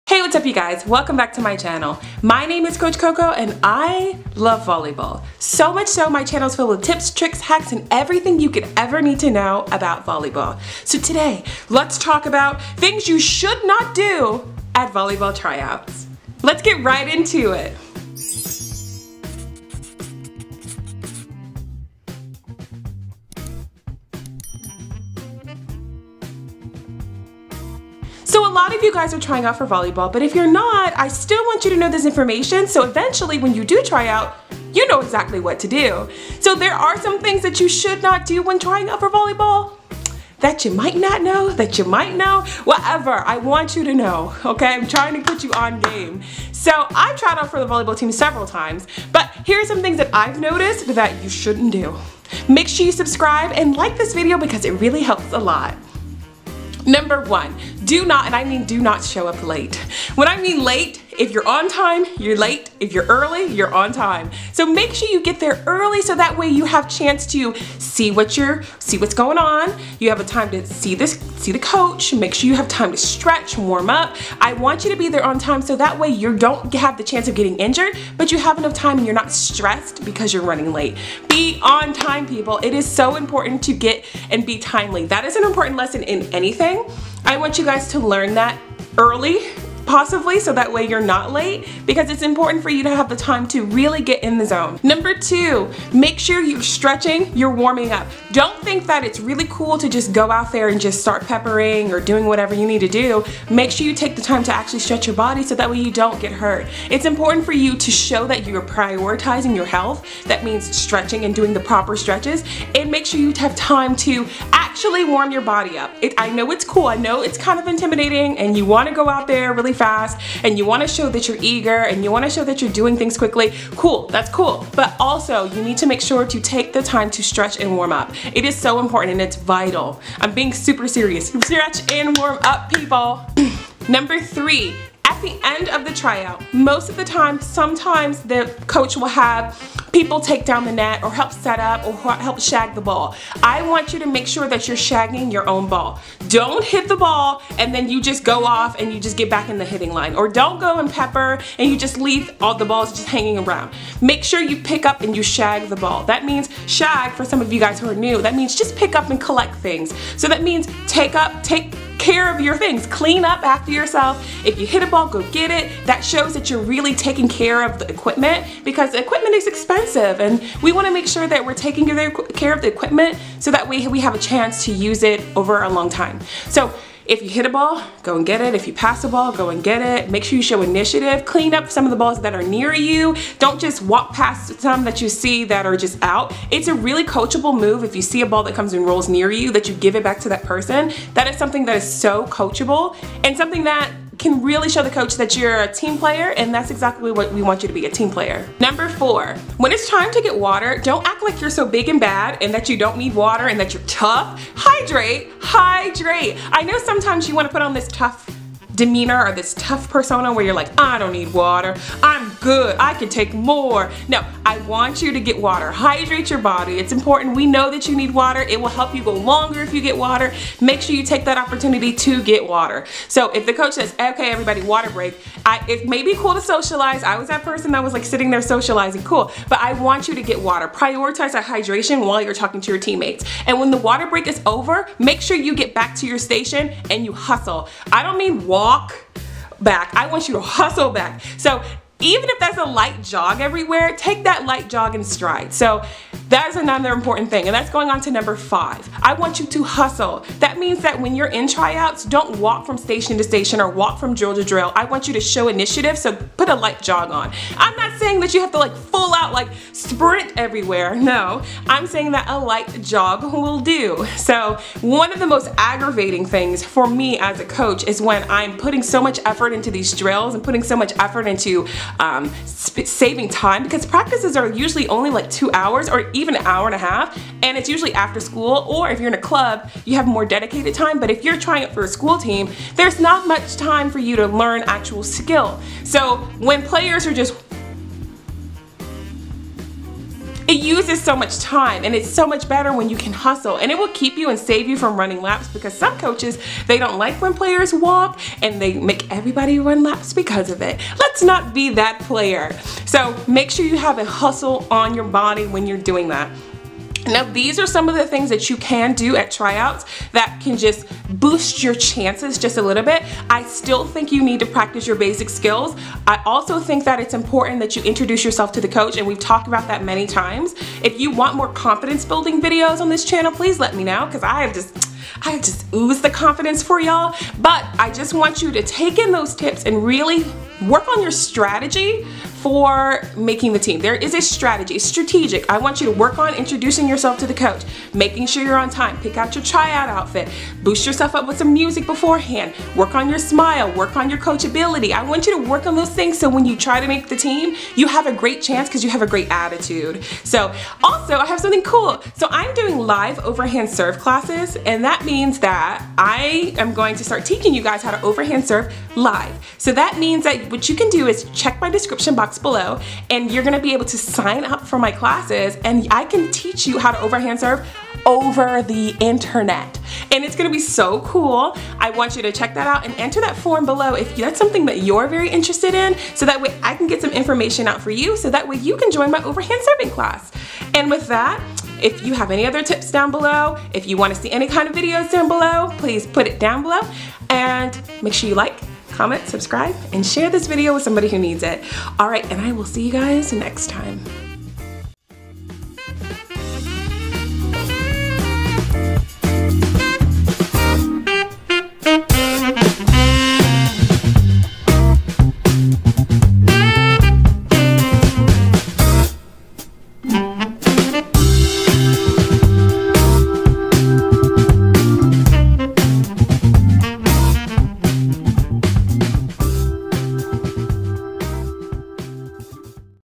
DON'T do this when you try out for Volleyball!!!
Etsy Shop With Volleyball Bracelets!

I know that so many of you have messaged me recently about your volleyball tryouts coming up! Well, let me tell you right now some things that you should not do when trying out for your team. I'm not even joking! Believe me on this one. I have tried out for the volleyball team more times than you can count and I have seen a variety of different things going on. I want you to do the best that you can, be confident, and not have to worry about any of the things on this list. The volleyball season is among us and even if you aren't trying out for your volleyball team, these tips can certainly still help.

△ My Favorite Spandex - [Great Selection Avaliable]

△ Beginner Knee Pads -

△ My Favorite Injury Prevention Tool